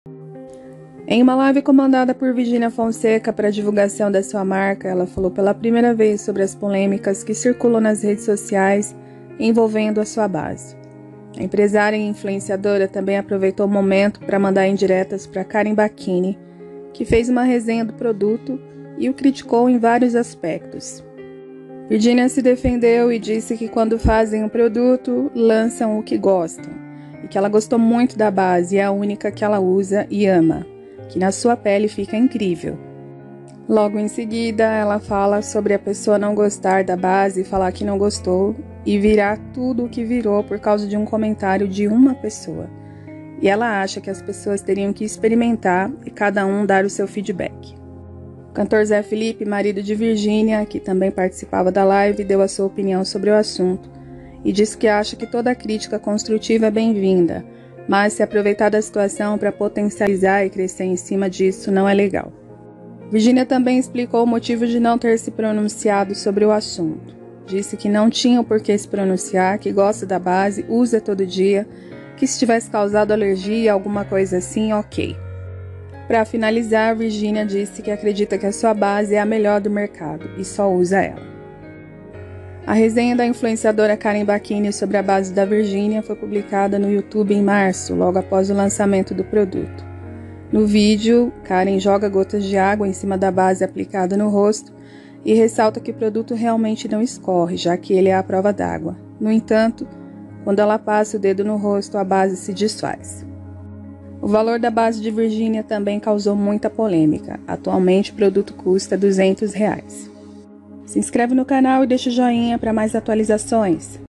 Virgínia Fonseca, Finalmente Quebra Silêncio e Expõe Realidade ao Vivo, Zé Felipe Não se Cala...
CANAL DE NOTICIAS URGENTES, VIDEOS TODOS OS DIAS